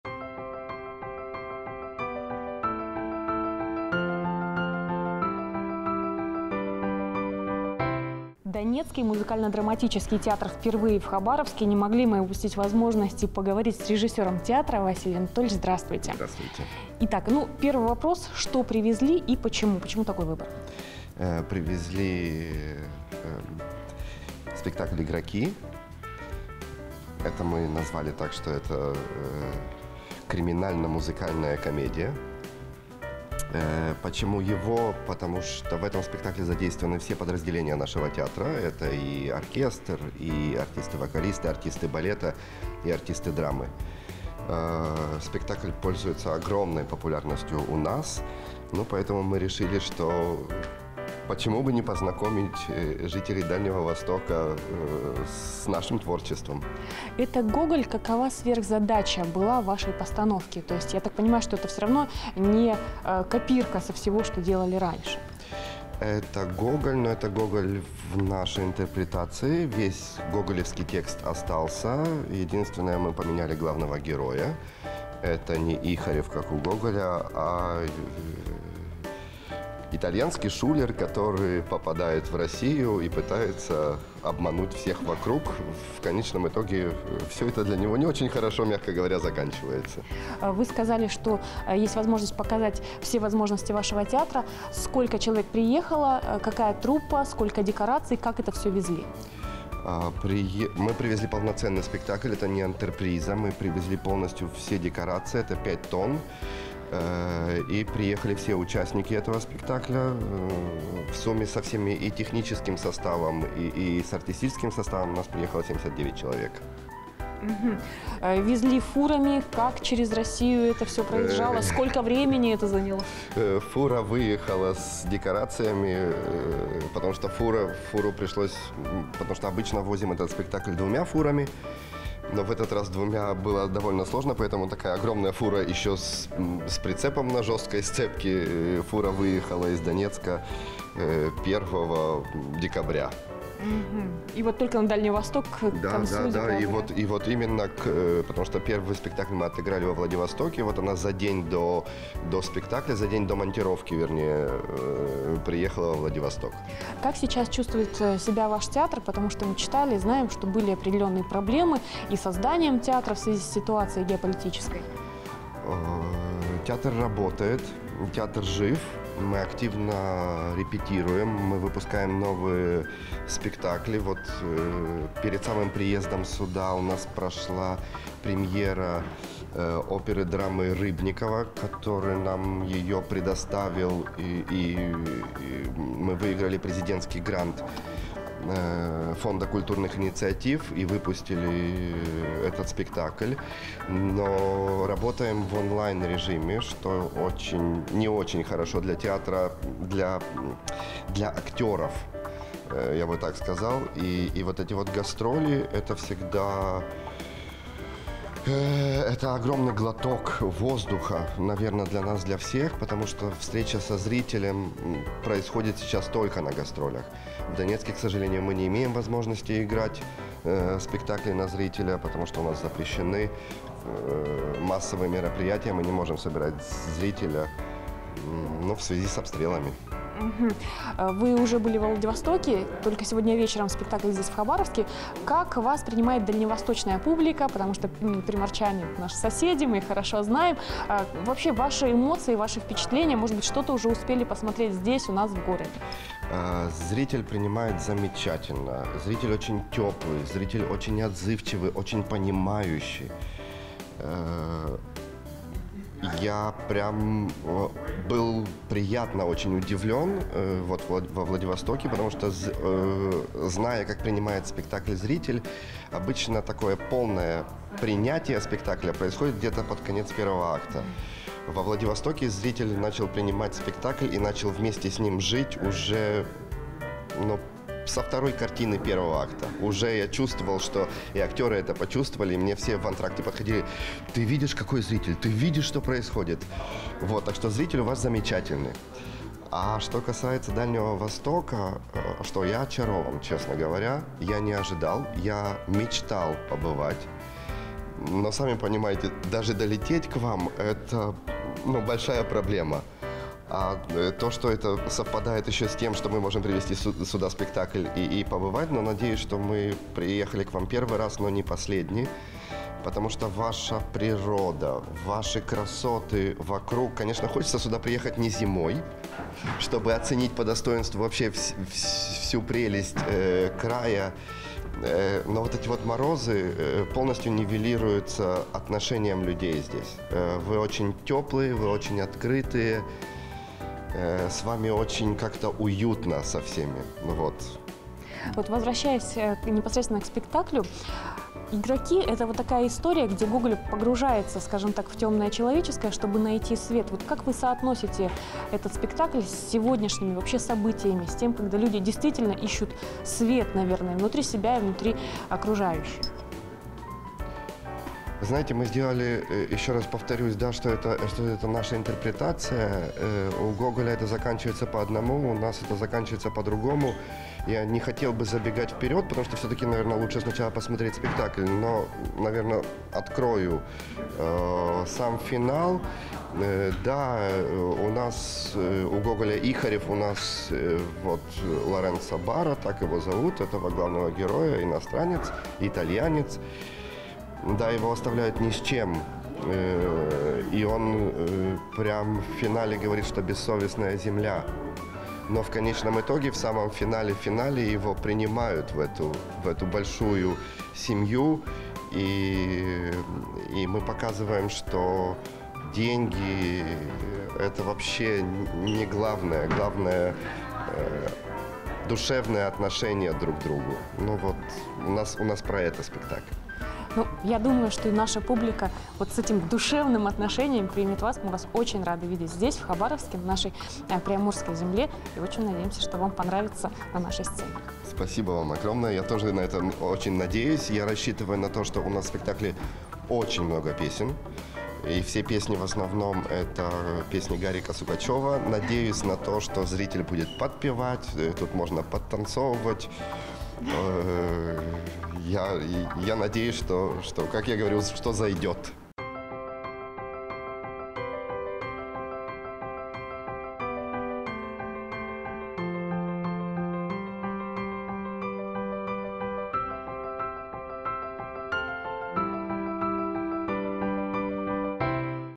Донецкий музыкально-драматический театр в Хабаровске / Хабаровск. Культурный
Донецкий музыкально-драматический театр приехал в Хабаровск. Благодаря проекту «Большие гастроли» труппа из почти 80 человек привезла для нашего зрителя спектакль «Игроки». Единственный показ пройдет 25 декабря.